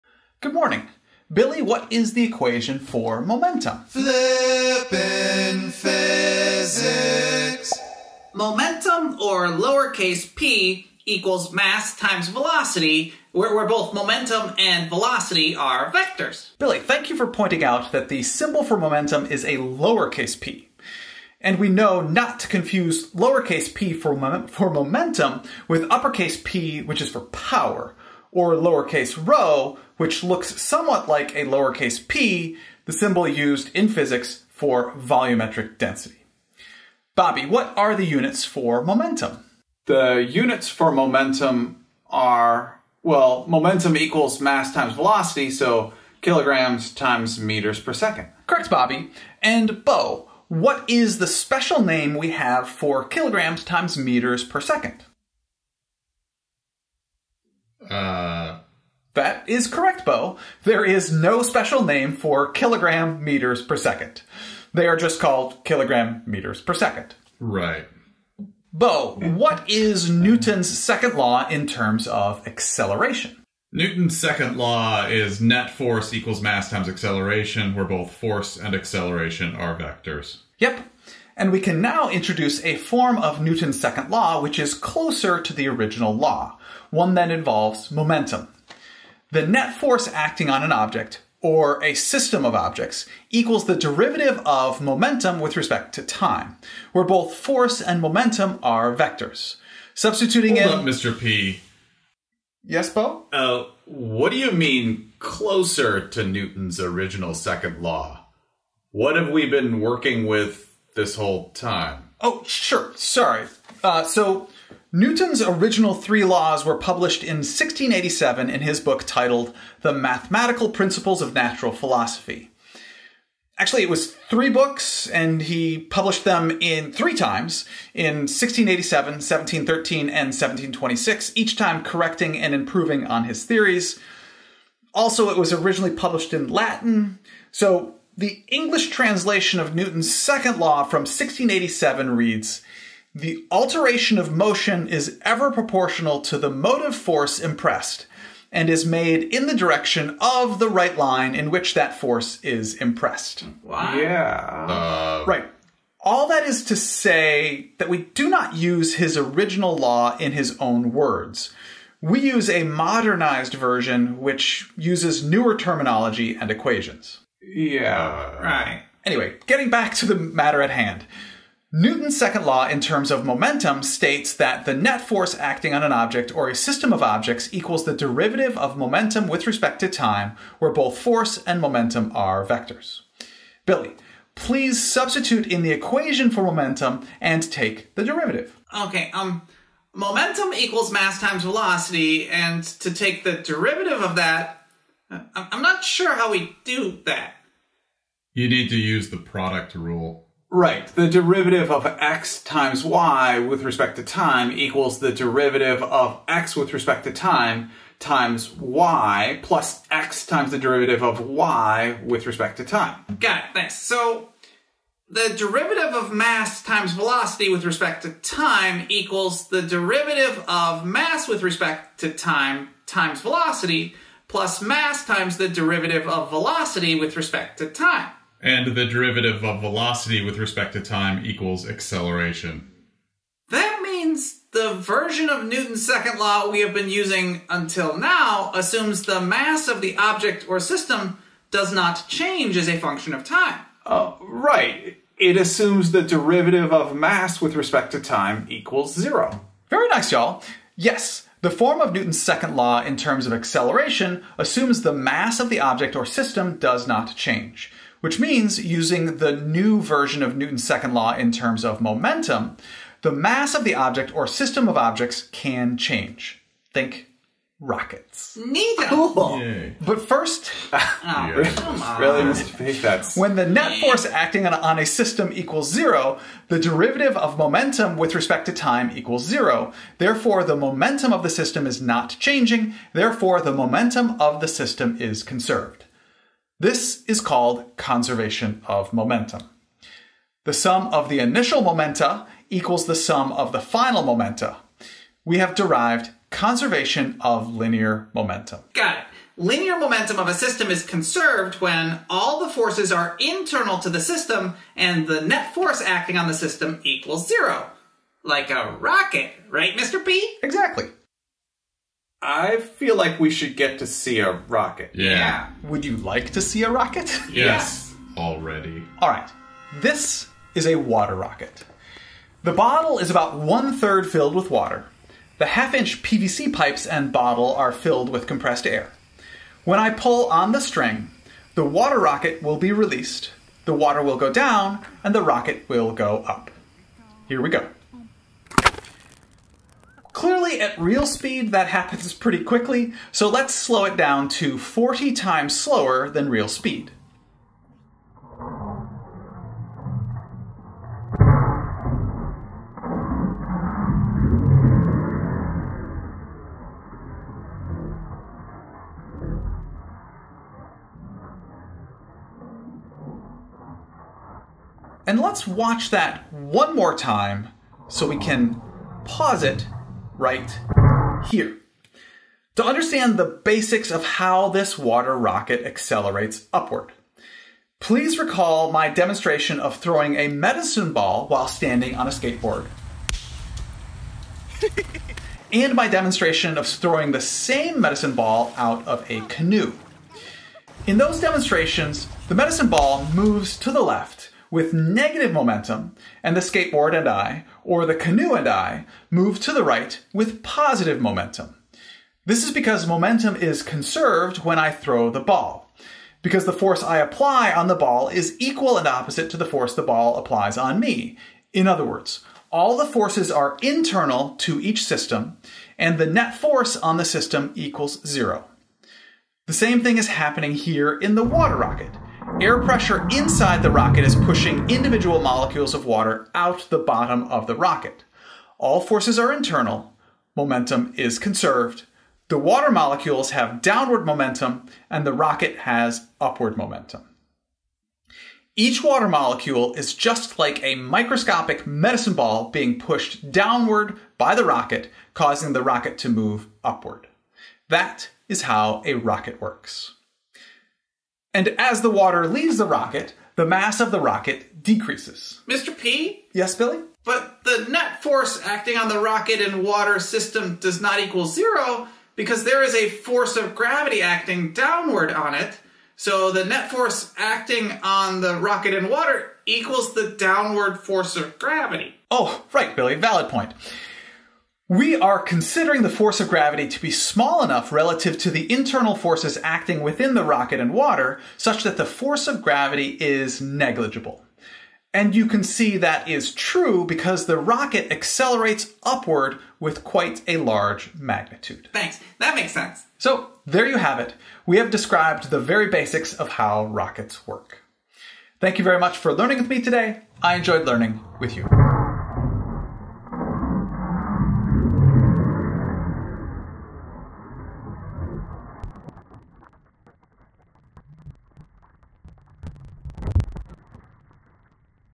Conservation of Momentum Derivation and Rocket Demonstration
Newton’s Second Law in terms of Momentum is introduced and used to derive Conservation of Linear Momentum. A water rocket in slow motion is used to demonstrate this as well. This is an AP Physics C: Mechanics topic.

Next Video: Introductory Conservation of Momentum Explosion Problem Demonstration

Previous Video: Introduction to Conservation of Momentum with Demonstrations

Thank you to Julie Langenbruner, John Paul Nichols, Scott Carter, and Adithya Bhaskara for being my Quality Control Team for this video.

Content Times:
0:00 Reviewing Momentum
1:08 Newton’s Second Law - Momentum Version
4:32 Conservation of Momentum Derivation
5:19 Rocket Demonstration

FYI: I combined instructions from these two sources to create my water rocket launcher:
Also, all of my 2 liter bottles had an opening which was to small to fit the 1/2” PVC pipe. So, I warmed the neck of the 2 liter bottle in boiling water and fitted it over the 1/2” PVC pipe to stretch the opening to fit. 😀